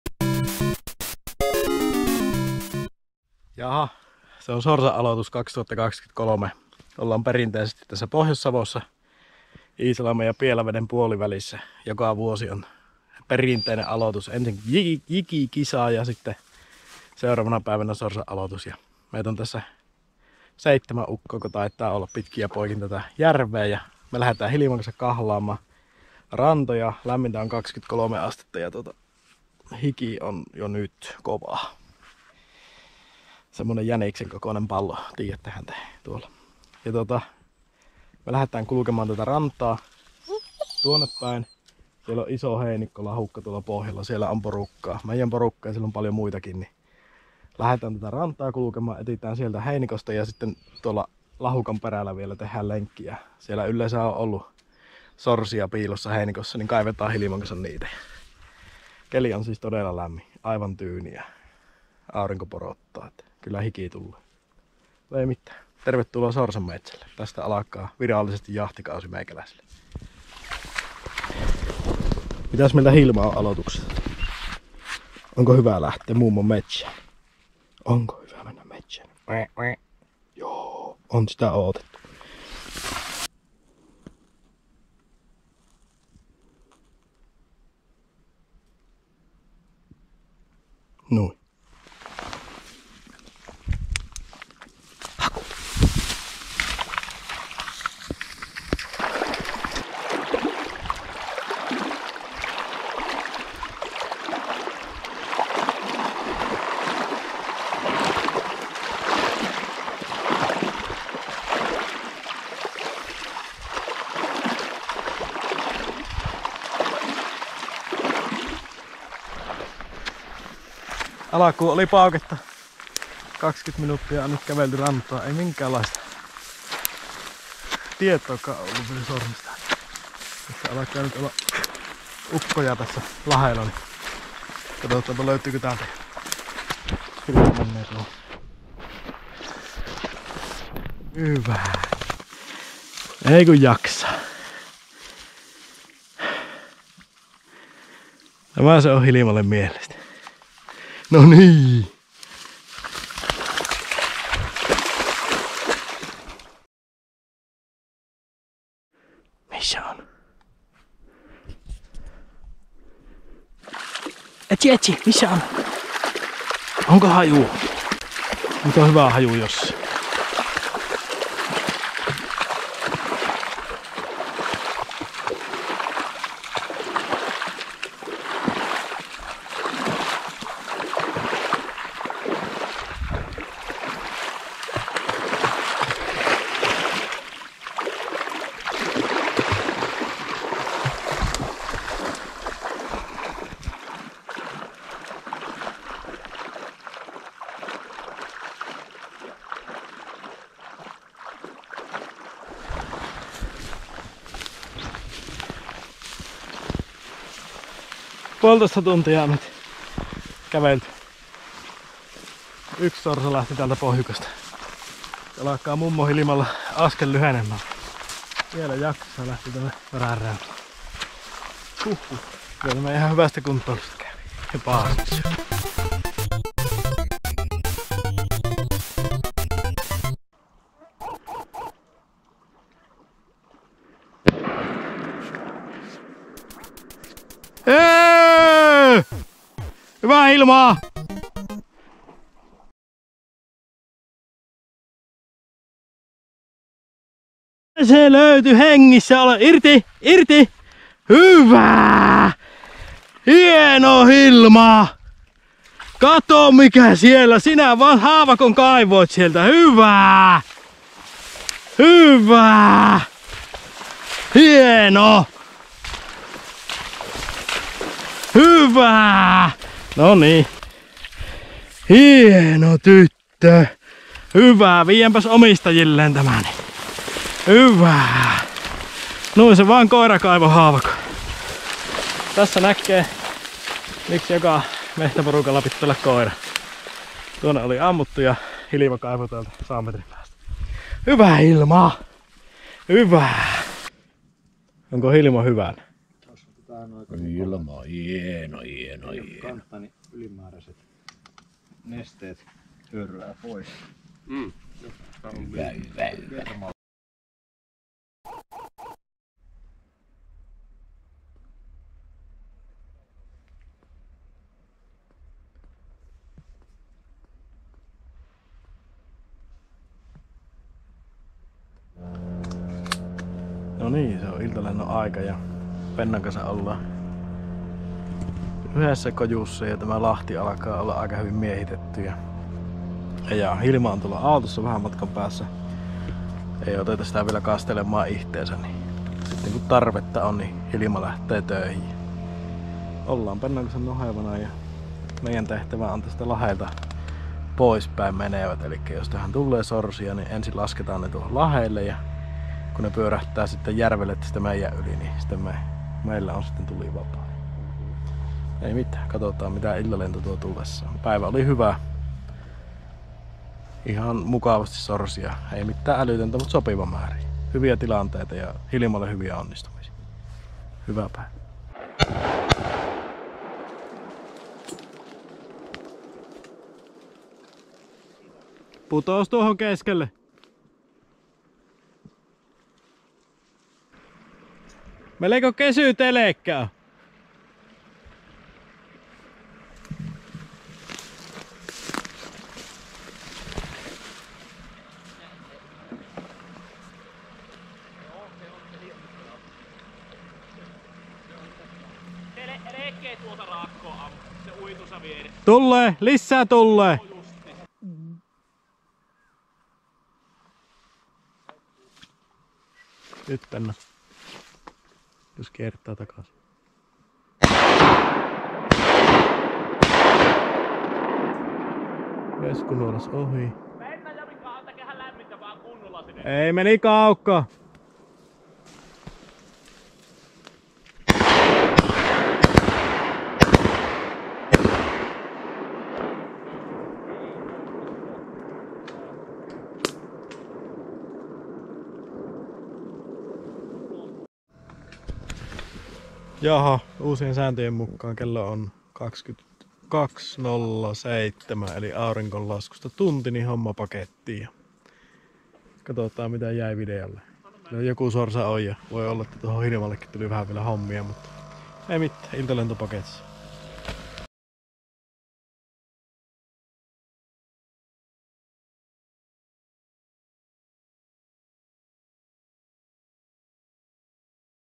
Sorsastuksen aloitus 20.8.2023 Savossa
JänkäJäppisen jahtikausi alkaa yleensä sorsastuksesta kun päästään Hilma the Weimarinseisojan kanssa kiertämään kaislikoita. Videolla satunnaisia fiiliksiä ja pauketta aloituspäivästä!

JänkäJätkän löytää instasta nimellä @jankajatka ja muistathan klikata myös tämän kanavan tilaukseen!